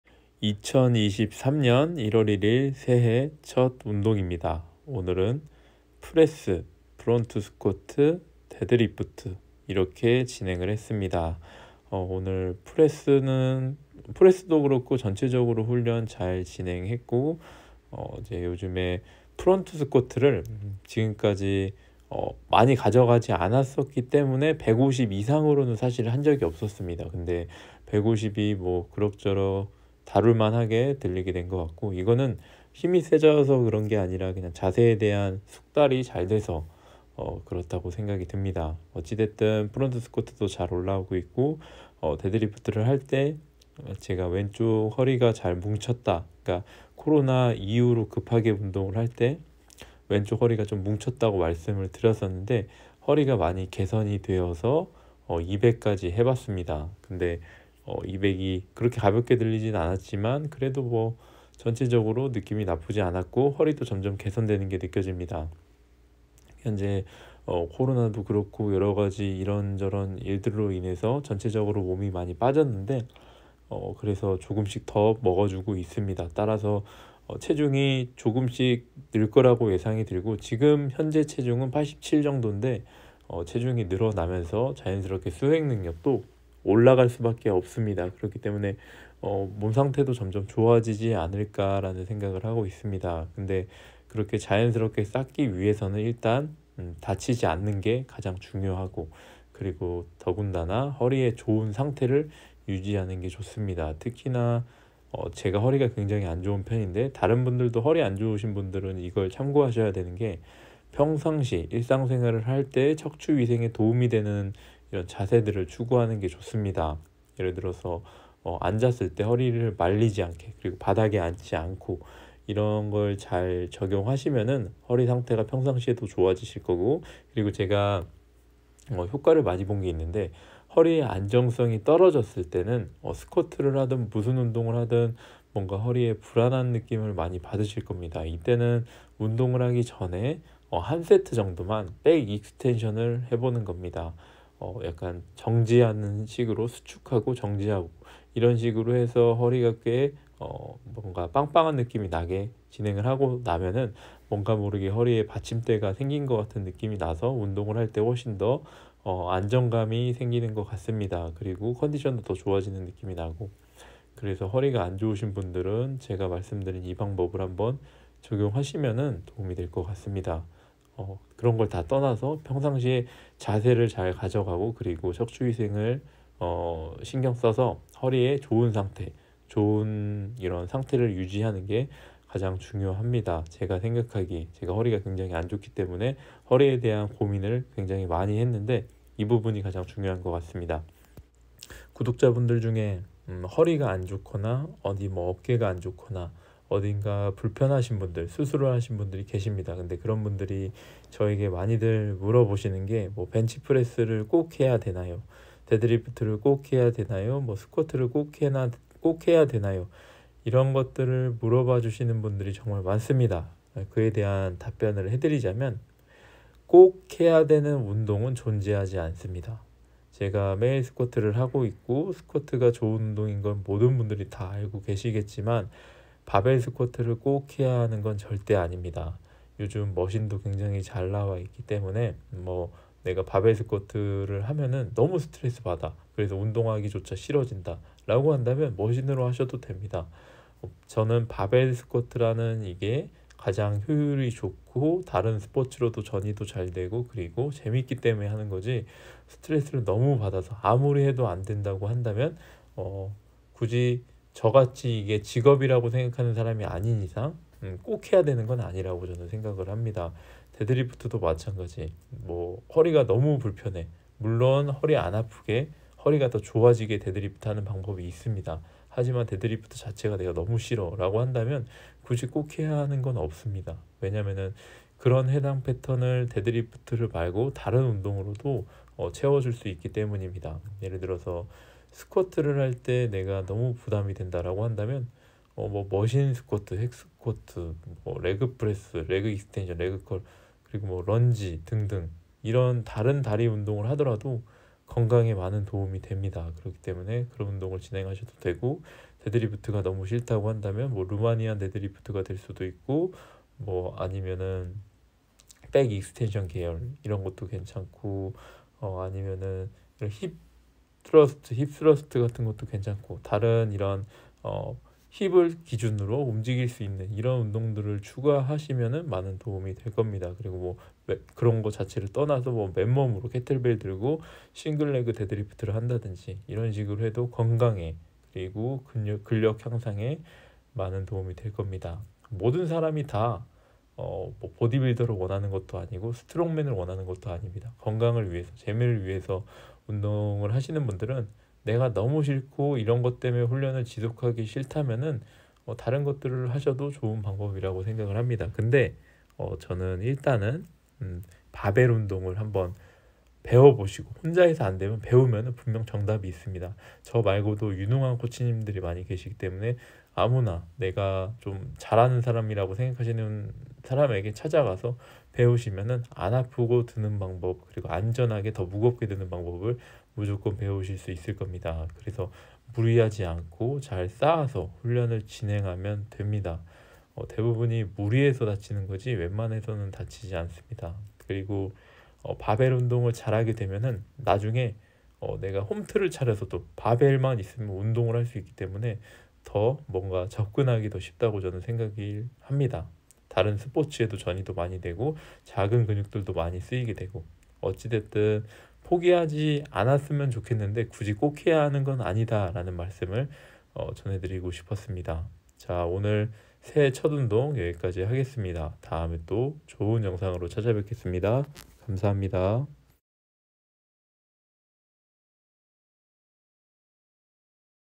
매일 스쿼트 405일차 : 꼭 해야 하는 운동이 있을까?(23.1.1. 일)
궁금한점은 댓글 OR 인스타그램

운동 정보 번역영상은 유튜브 검색 : 라이노스트렝스 - 운동정보